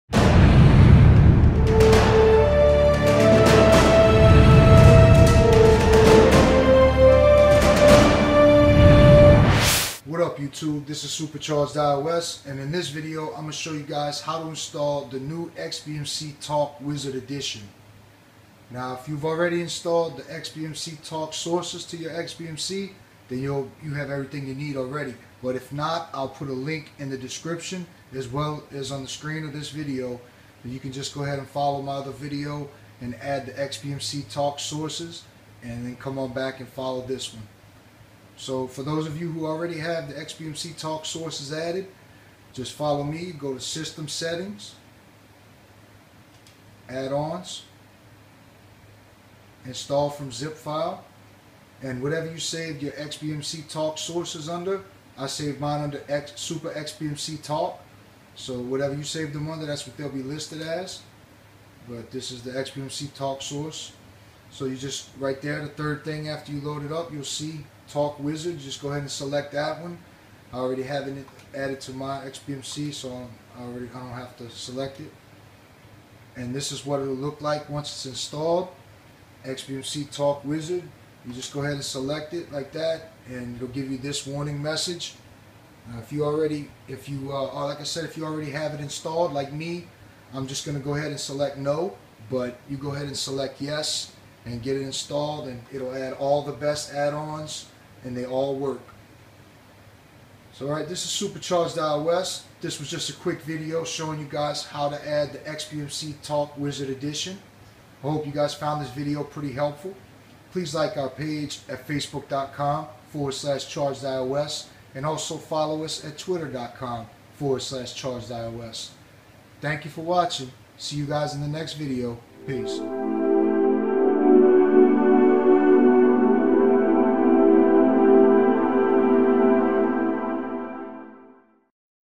How to install The 'XBMC TALK WIZARD EDITION' to your XBMC
This video shows you how to install the new XBMC Talk Wizard Edition to your XBMC

*MOST OF THE BEST ADD-ONS LIKE MASHUP, 1CHANNEL, MAINTENANCE TOOLS, ETC. ARE NOW AVAILABLE FROM SUPER-REPO*